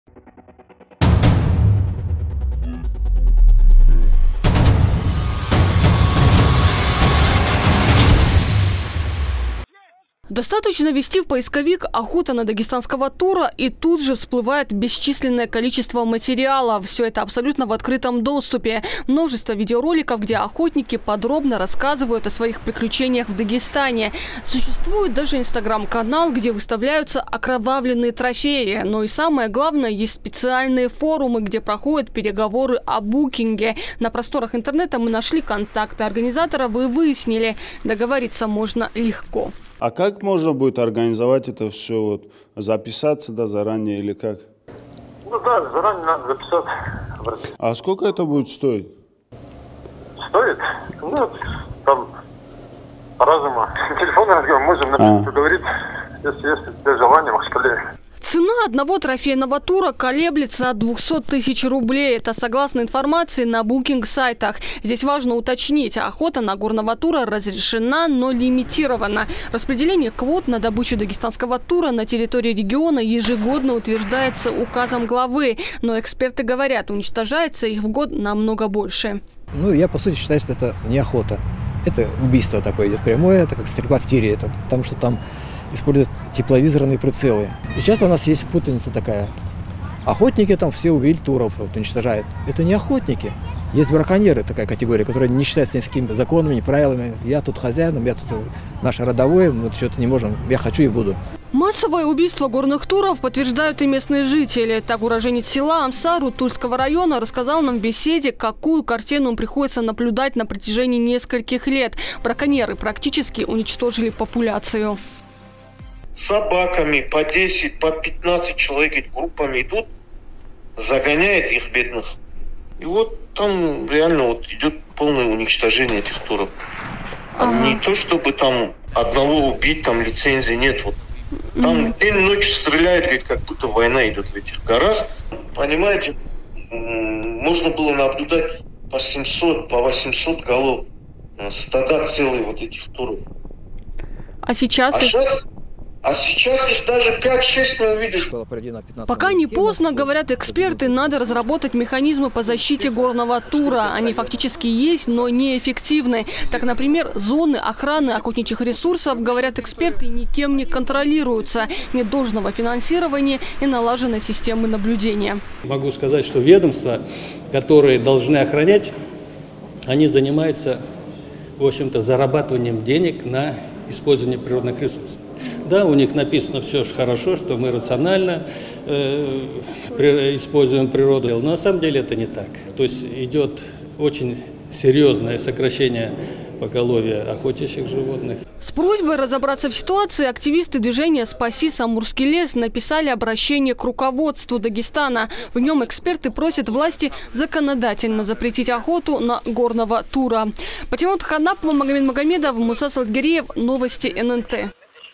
В Дагестане призывают законодательно запретить охоту на горного тура
Безжалостная охота ради хвастовства? Активисты Дагестана требуют законодательно запретить убивать дагестанского тура. Из-за желания наживы или просто чьей-то забавы отдельные животные в нашей республике уже практически исчезли. По мнению ученых, вскоре мы рискуем потерять и горного тура. В проблеме разбиралась моя коллега Патимат Ханапова.